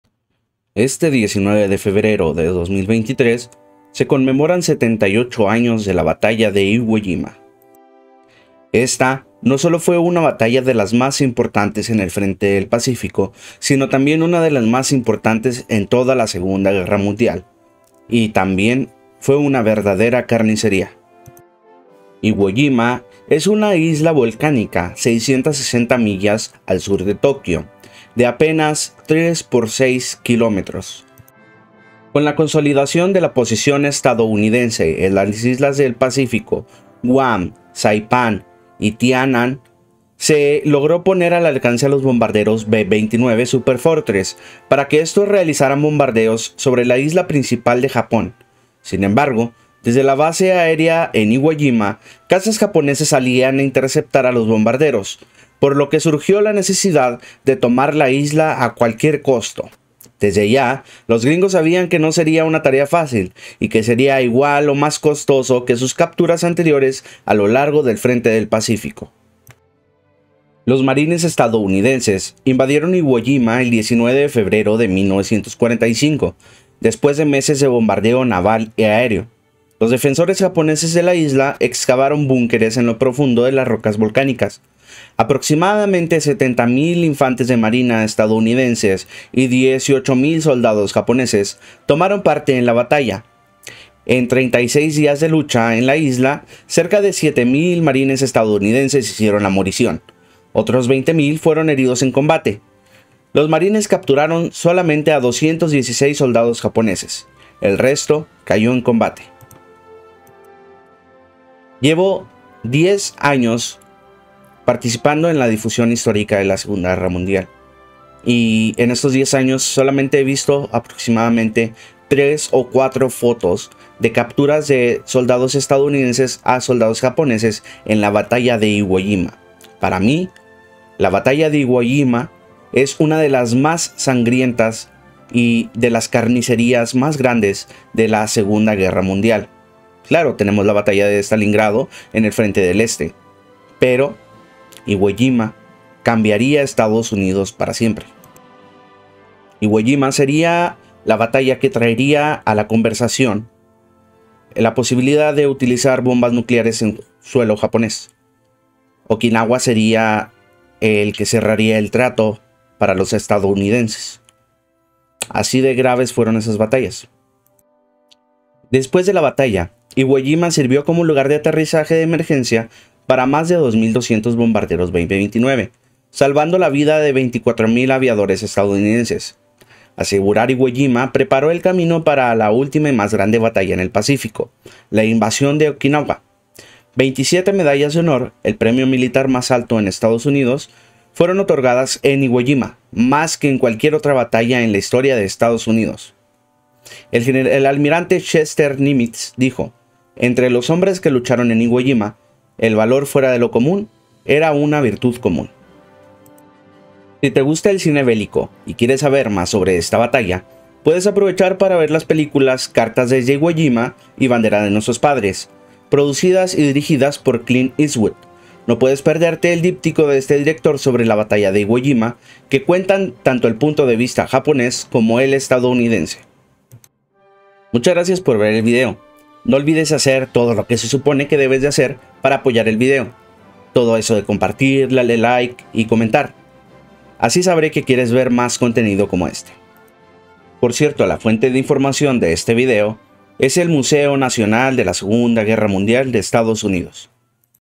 78 AÑOS DE LA Batalla de Iwo Jima una de las batallas mas s4ngrient4s de la SGM
Batalla de Iwo Jima